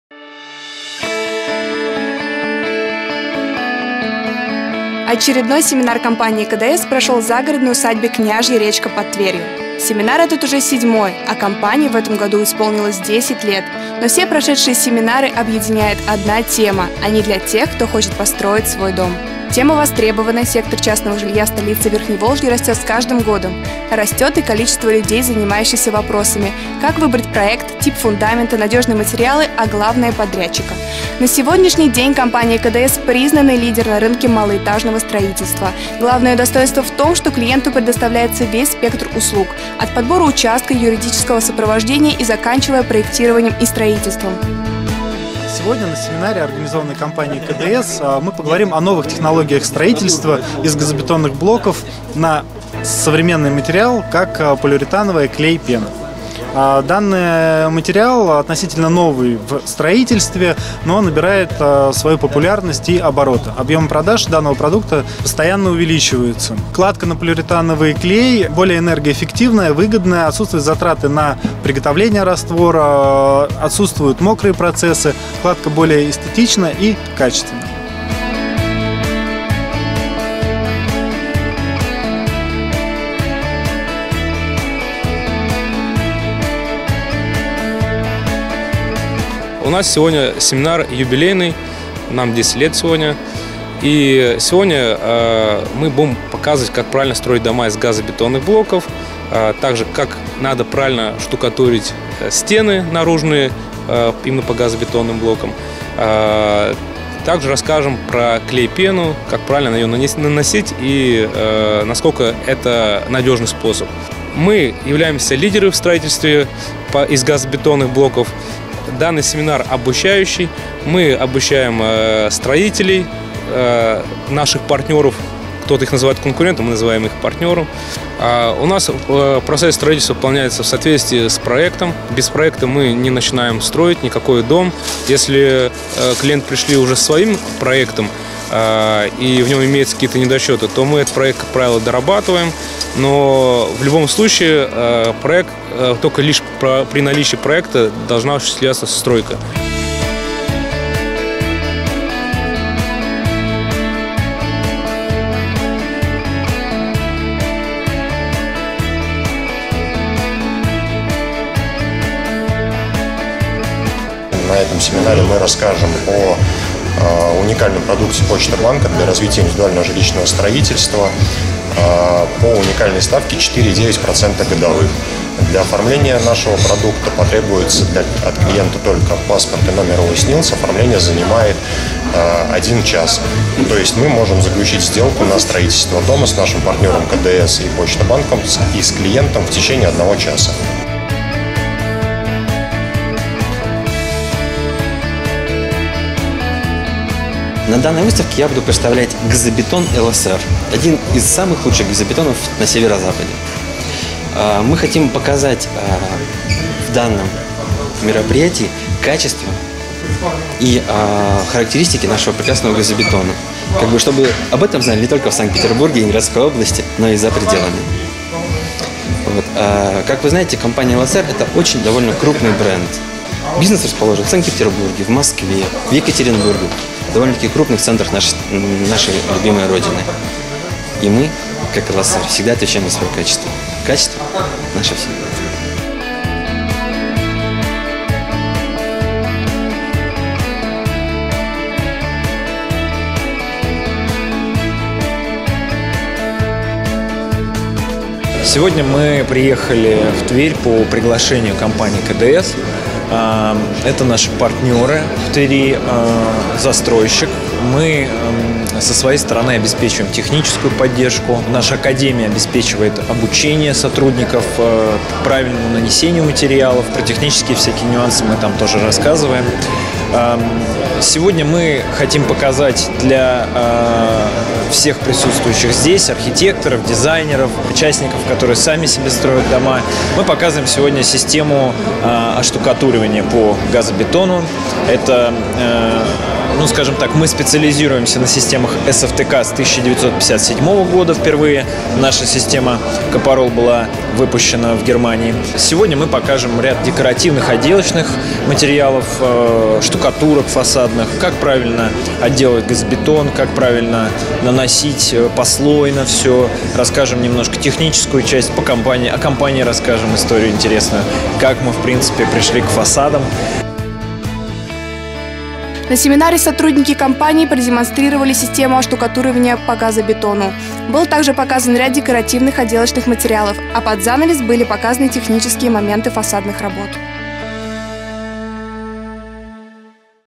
«КДС» поможет жителям Тверской области построить настоящие дома
На базе отдыха «Княжья речка» под Тверью прошел семинар для тех, кто хочет жить в комфорте.